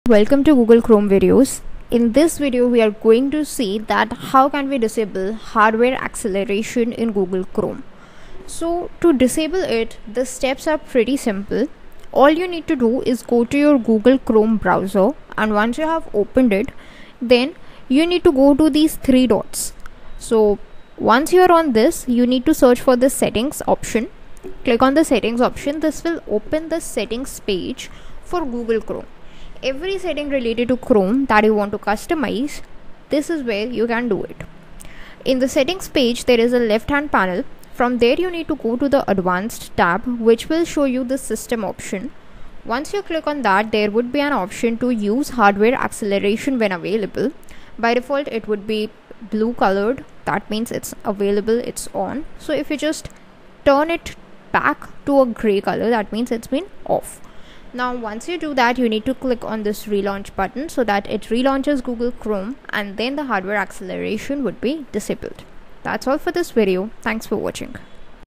How to Enable or Disable Hardware Acceleration in Chrome
How To Disable Hardware Acceleration In Google Chrome | Turn Off GPU Hardware Acceleration Chrome  in Windows 10 / Windows 11